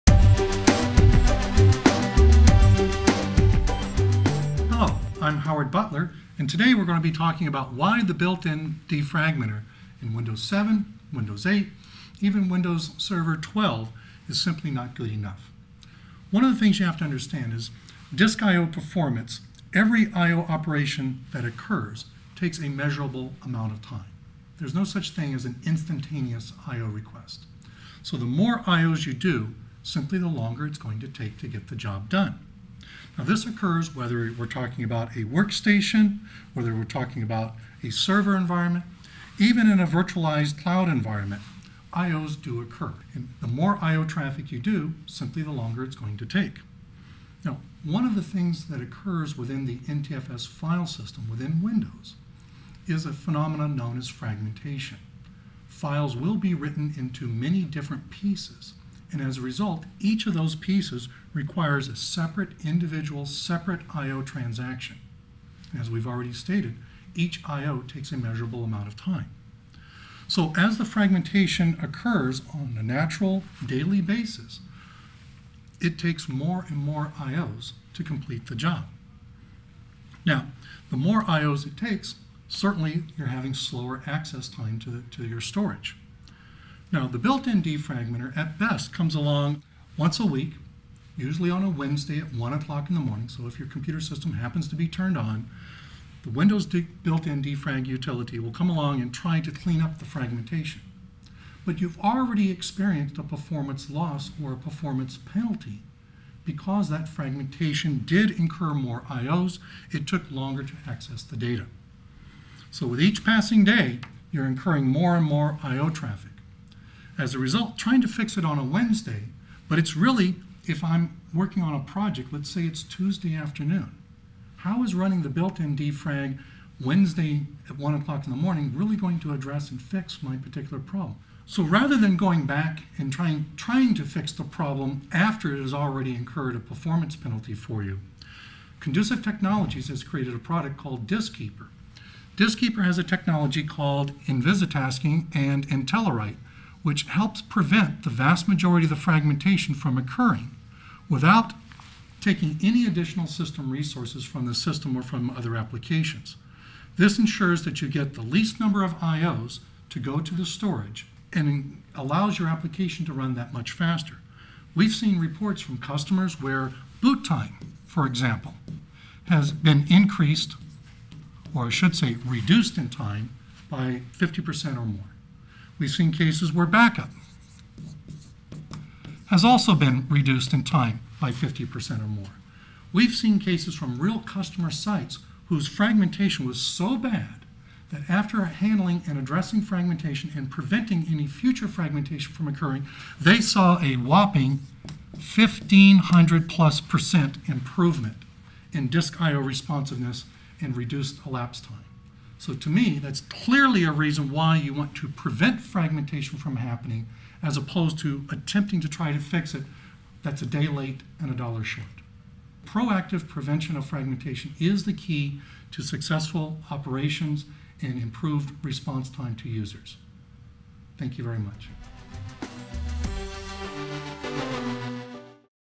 Why Windows Built-in Defrag is Not Good Enough
Learn why the Built-in Defrag in Windows 7, 8 and Server 2012 is simply not good enough. This "Quick take" video demonstrates how running the built-in defrag after your system has already experienced a performance issue, doesn't address the real problem. Learn how Diskeeper uses proactive prevention of fragmentation to help customers speed up boot-up and back up times by 50% and improve I/O responsiveness by 1500+%.